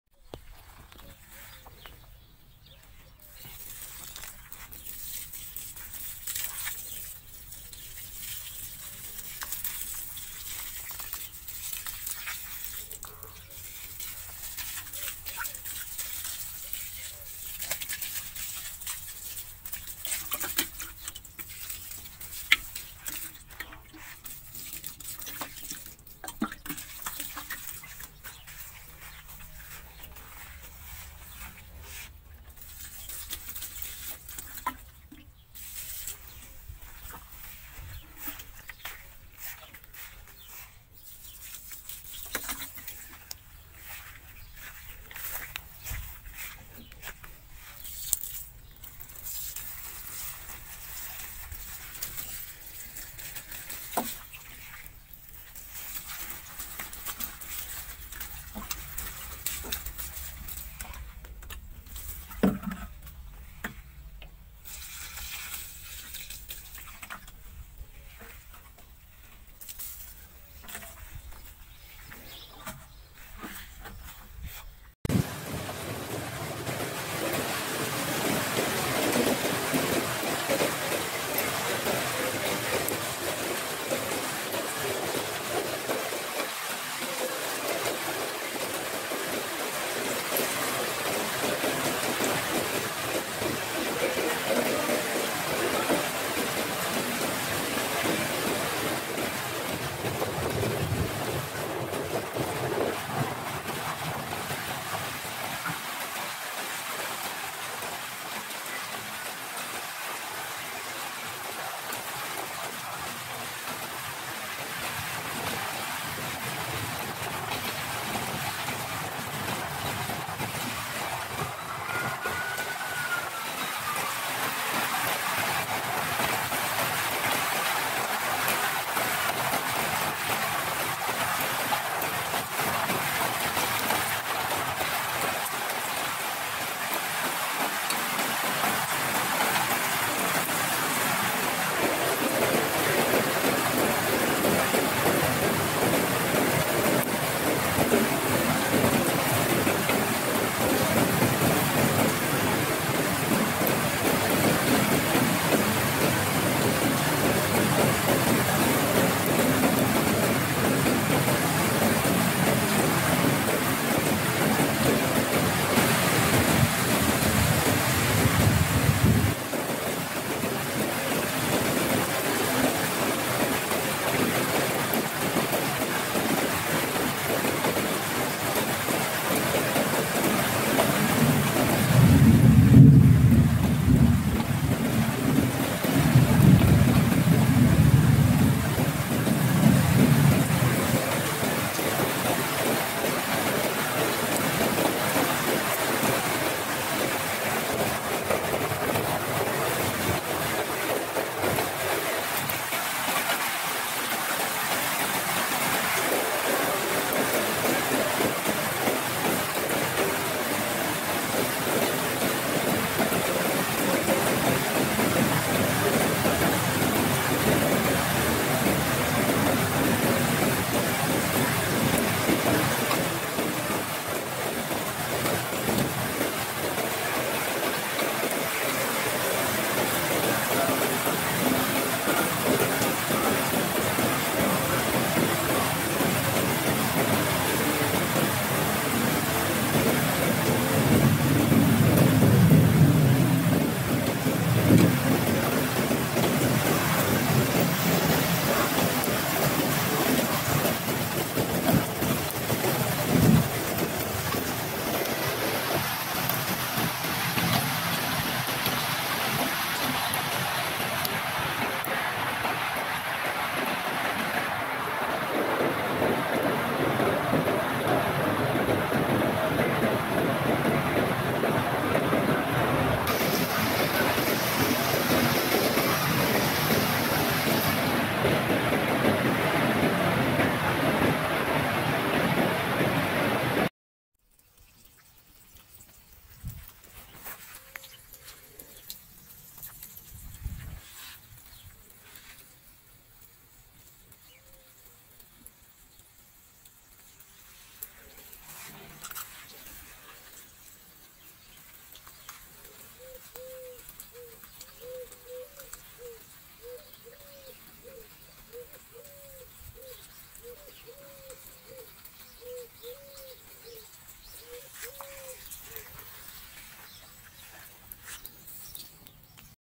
ASMR UNINTENTIONAL | Water sound (watering, rain, storm) 🐳
To be honest, the watering part was not filmed by me but by my father who, as you can see, has trouble doing two things at the same time!

Originally, he just wanted to show us that he hadn't forgotten to water the plants, and I'm glad to see that he thought of it and that at the same time, it gives us a great unintentional relaxing video!